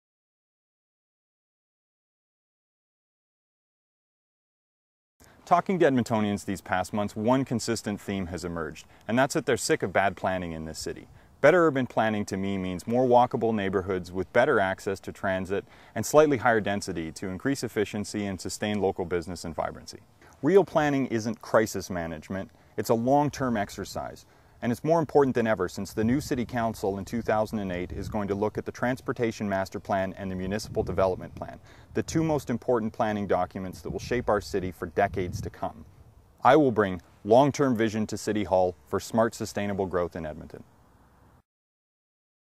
Put Urban Planning First
Don Iveson, Edmonton City Council Candidate for Ward 5, outlines the importance of planning better for the future of Edmonton.

On October 15th, 2007, Vote Don Iveson for Ward 5 City Councillor.